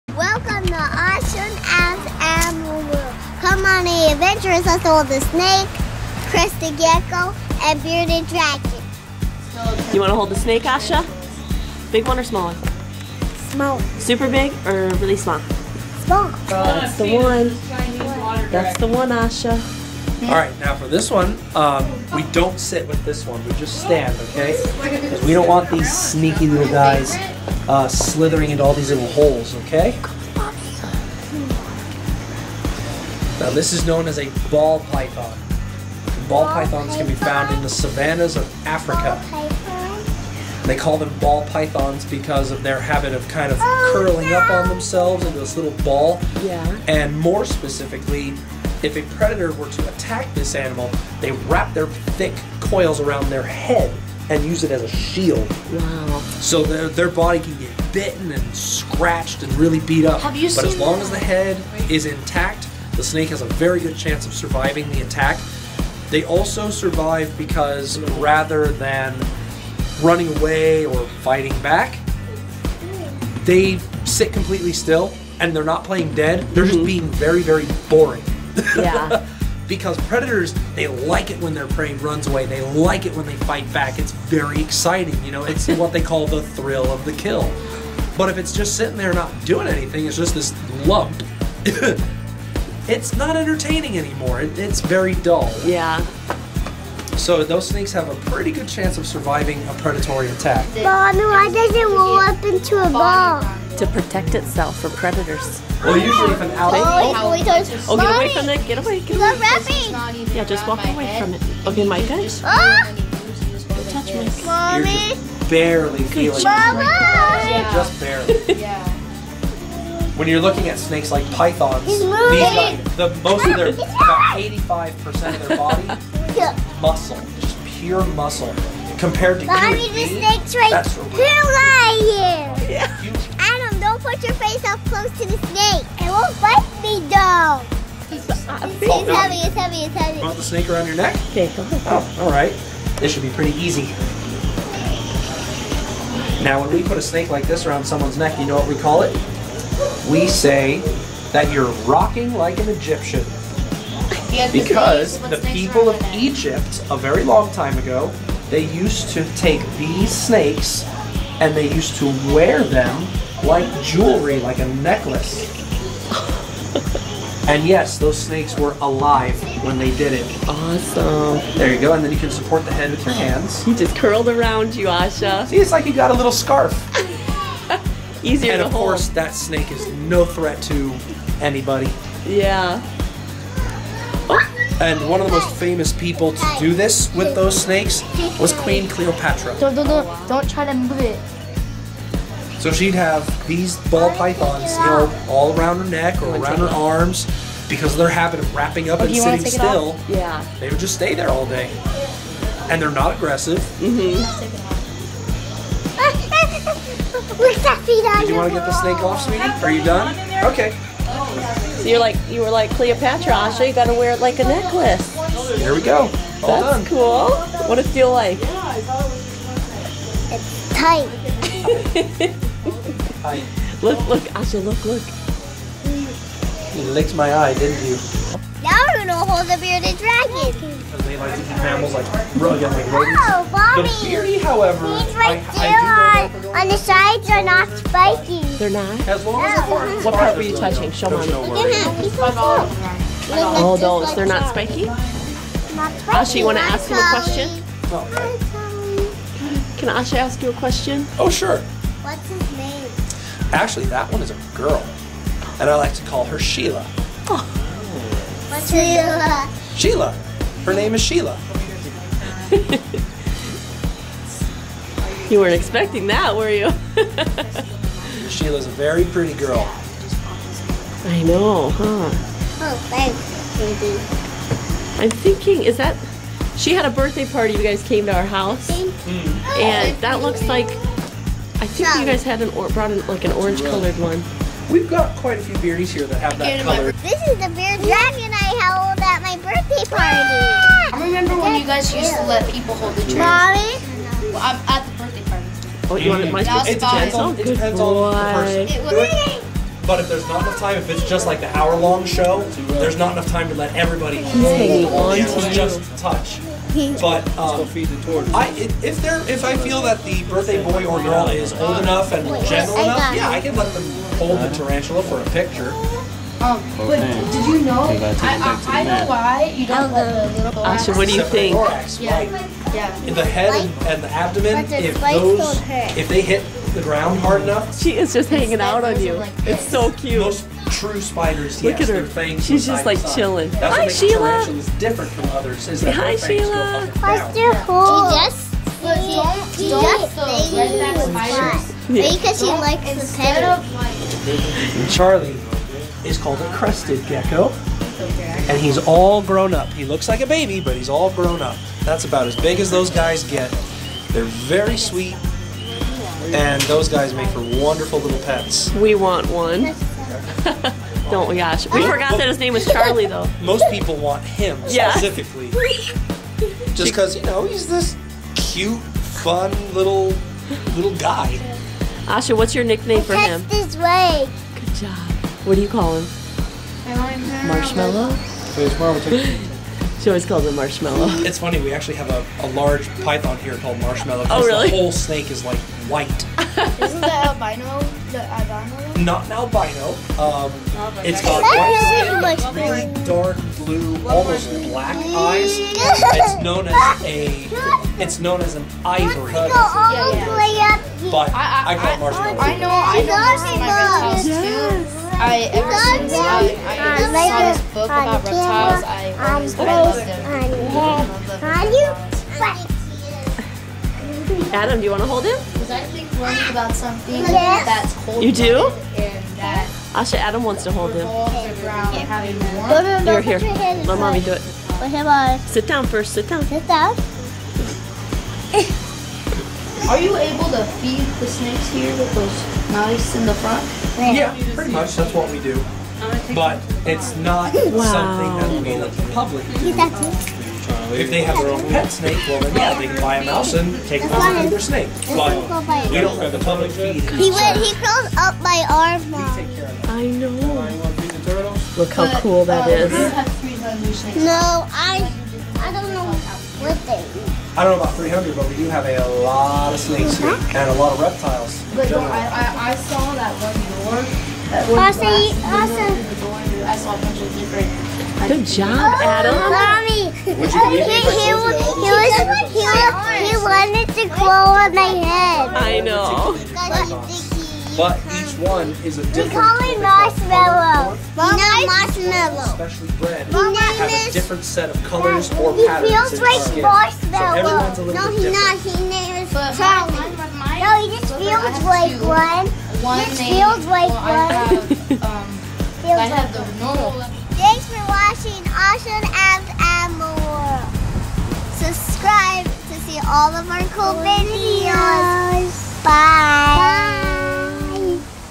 REPTILE ZOO! SNAKE Wraps Around Asha, KIDS Animal Adventure, Bearded Dragon, Crested Gecko
Come on an animal adventure with us to the Reptile Zoo (& Prehistoric Pets) in Fountain Valley, CA. A Ball Python snake wraps around Asha's neck, Adam holds a crested gecko, and Asha loves on a bearded dragon. Learn a lot of cool facts about the lizards and snakes from their reptile expert and enthusiast.